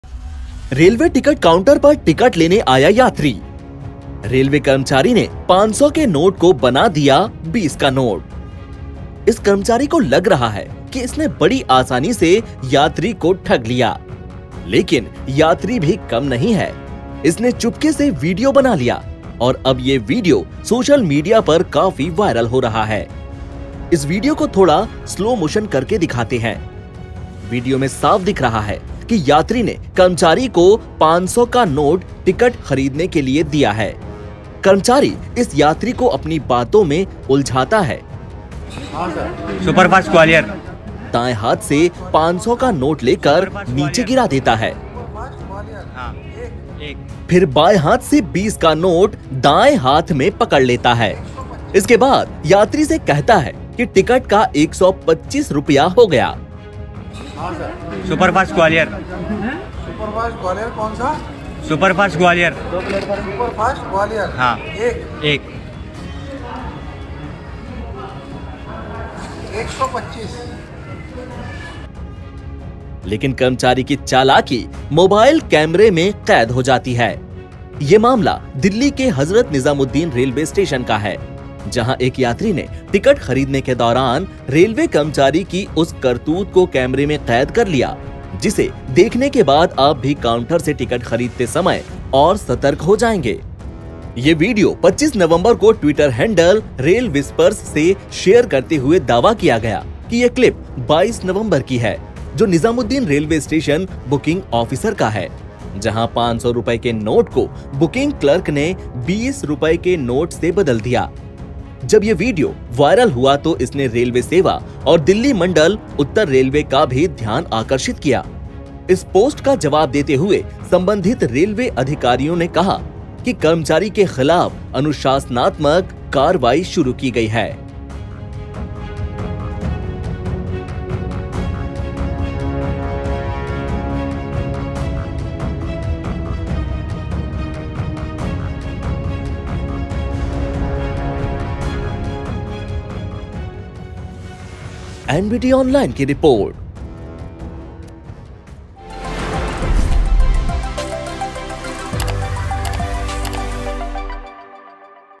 Viral Video: Hazrat Nizamuddin Railway Station Ticket Counter पर Railway Employee ने बदला नोट
Viral Video: Hazrat Nizamuddin Railway Station Ticket Counter पर Railway Employee ने बदला नोट | Indian Railway

रेलवे टिकट काउंटर पर टिकट लेने आया यात्री... रेलवे कर्मचारी ने 500 के नोट को बना दिया 20 का नोट... इस कर्मचारी को लग रहा है कि इसने बड़ी आसानी से यात्री को ठग लिया। लेकिन यात्री भी कम नहीं है। इसने चुपके से वीडियो बना लिया और अब ये वीडियो सोशल मीडिया पर काफी वायरल हो रहा है। इस वीडियो को थोड़ा स्लो मोशन करके दिखाते हैं... वीडियो में साफ दिख रहा है कि यात्री ने कर्मचारी को 500 का नोट दिया है। कर्मचारी इस यात्री को अपनी बातों में उलझाता है। दाएं हाथ से 500 का नोट लेकर नीचे गिरा देता है, फिर बांए हाथ से 20 का नोट हाथ में पकड़ लेता है। इसके बाद यात्री से कहता है कि टिकट का 125 रुपया हो गया। लेकिन कर्मचारी की चालाकी मोबाइल कैमरे में कैद हो जाती है। ये मामला दिल्ली के हजरत निजामुद्दीन रेलवे स्टेशन का है, जहां एक यात्री ने टिकट खरीदने के दौरान रेलवे कर्मचारी की उस करतूत को कैमरे में कैद कर लिया, जिसे देखने के बाद आप भी काउंटर से टिकट खरीदते समय और सतर्क हो जाएंगे। ये वीडियो 25 नवंबर को ट्विटर हैंडल Rail Whispers से शेयर करते हुए दावा किया गया कि ये क्लिप 22 नवंबर की है, जो निजामुद्दीन स्टेशन बुकिंग ऑफिस का है। जहां 500 रुपये के नोट को बुकिंग क्लर्क ने 20 रुपये के नोट में बदल दिया गया। जब ये वीडियो वायरल हुआ तो इसने रेलवे सेवा और दिल्ली मंडल, उत्तर रेलवे का भी ध्यान आकर्षित किया। इस पोस्ट का जवाब देते हुए, संबंधित रेलवे अधिकारियों ने कहा, 'कर्मचारी के खिलाफ अनुशासनात्मक कार्रवाई शुरू की गई है।'